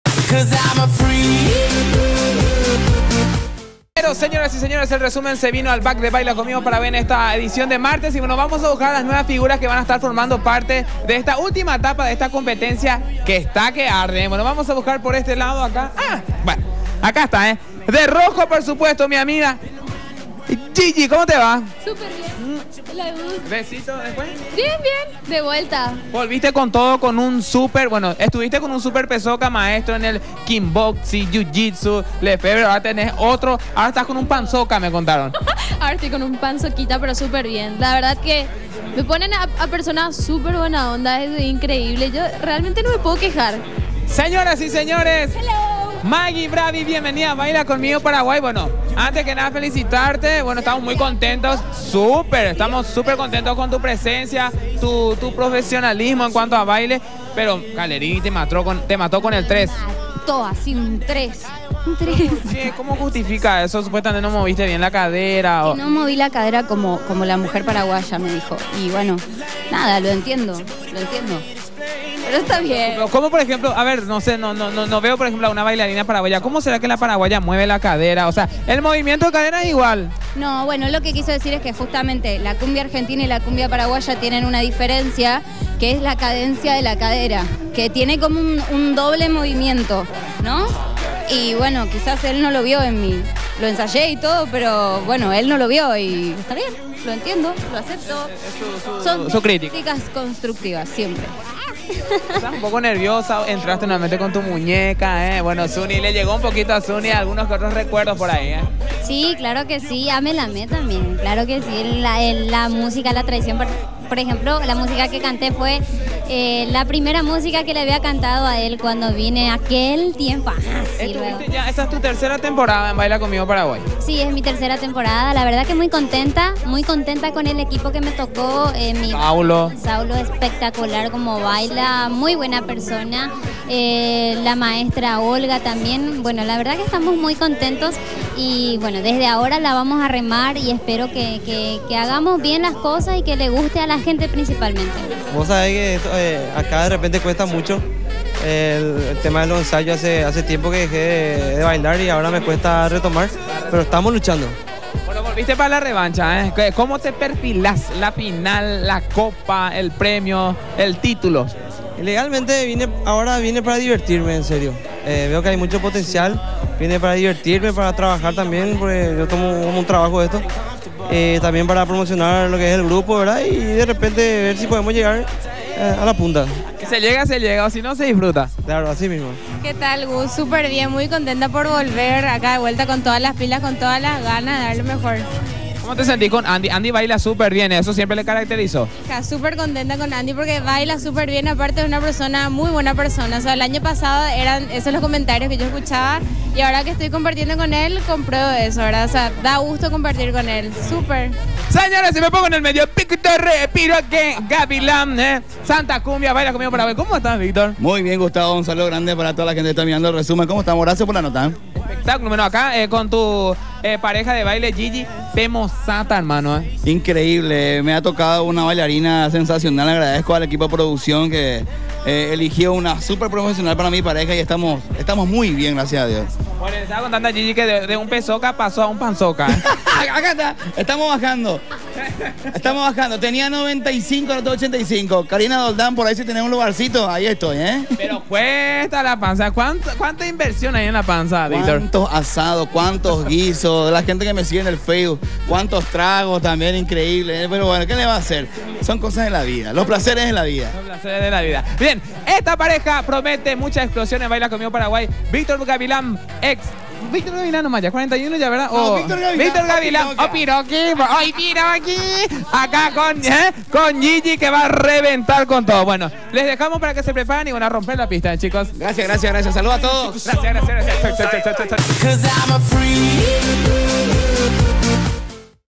Backstage con Gustavo - ¡El BCPY se pone mas bravo que nunca!
¡Mira el último Backstage que hicimos de la mano de Gustavo Corvalan donde entrevistamos a los nuevos participantes de la competencia que se pone mas caliente que nunca!